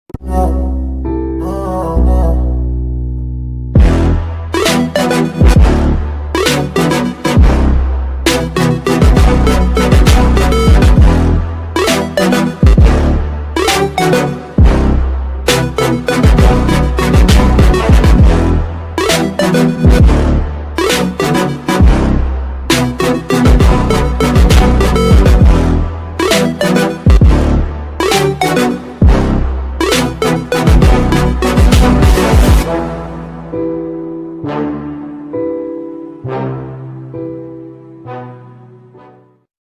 Painted greys//ORIGINAL MEME//Remake//X-Cat
I left the gacha community so I wanted to animate it instead, if you made this meme or want to, please update your description to change it to the NEW one (this one) hope ya understand, the song will be in the description of the old one